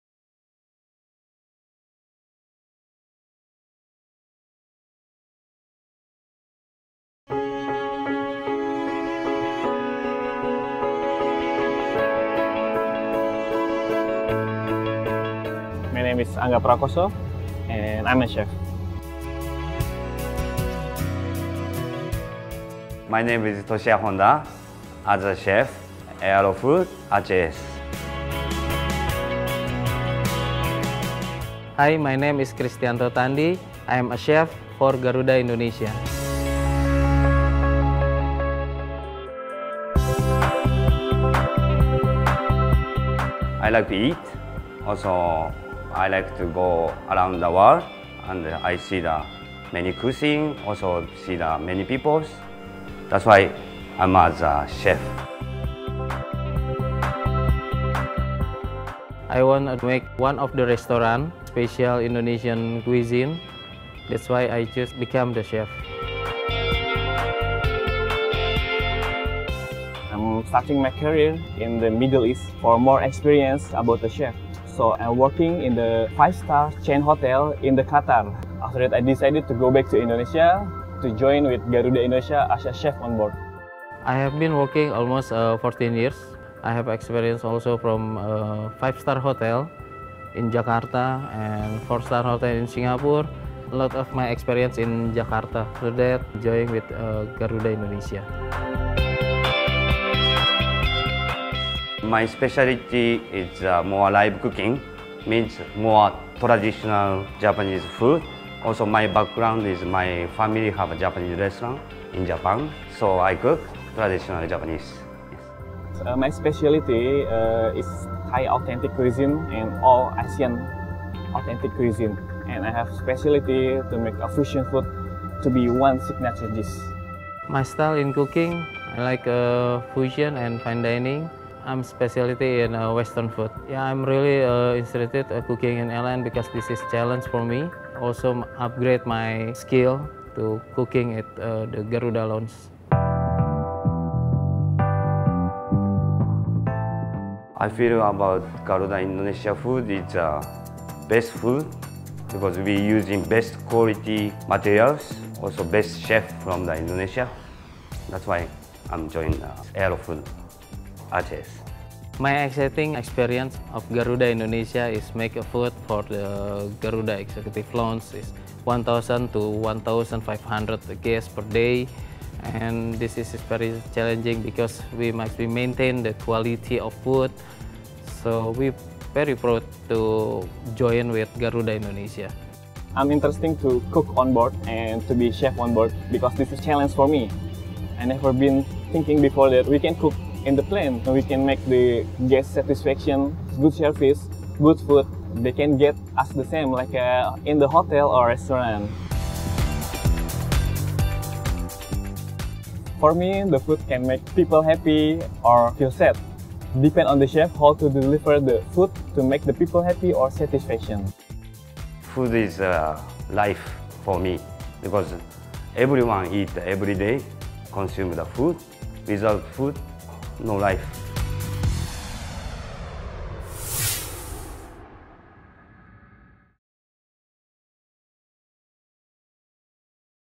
Garuda Indonesia - The Men Behind Our Culinary Delight
The food and beverages that we serve you each flight is made of natural ingredients with an international taste. We offer you many menus from our best chefs.